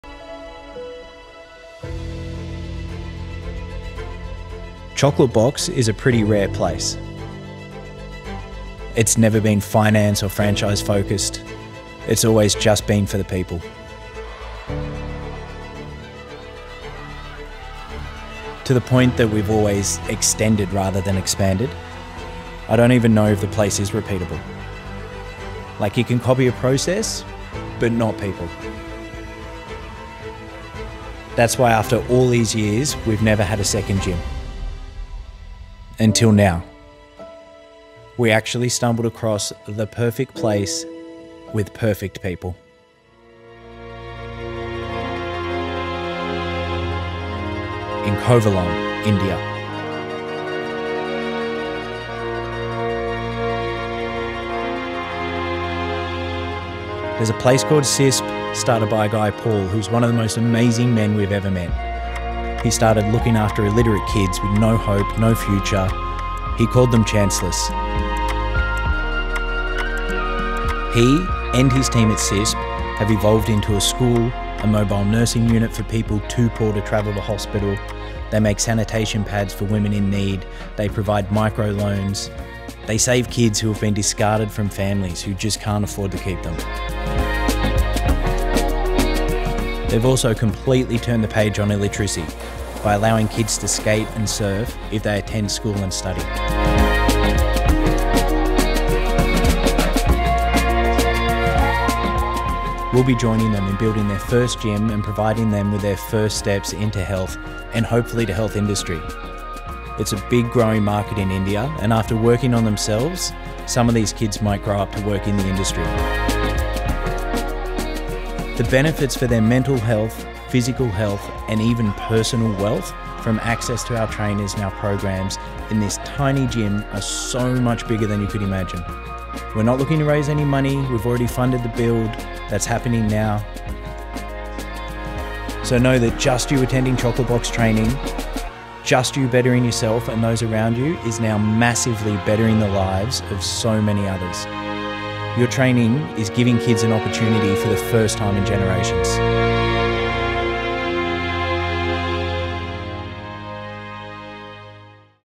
Chocolate Box Training - OUR NEW GYM
Chocolate Box Training is expanding, after 15 years we are opening the doors to our new space, in a brand new place.

We know it would be so hard to replicate our gym because of the people that make it such a special place.
However, we stumbled across the perfect people in the perfect place.
In Kovalam, India.

Paul and his team have been running SISP and helping kids in Poverty for over 20 years. The difference they have made to individual lives, and a community is amazing and Chocolate Box Training is proud to help play a small role in what they do.